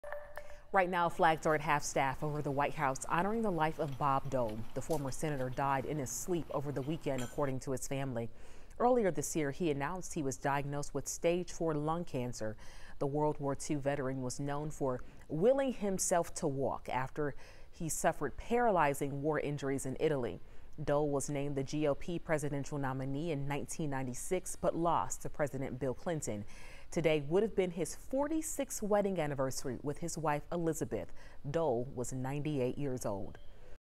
Remembering the life of Bob Dole
Monday would have been his 46th wedding anniversary with his wife Elizabeth.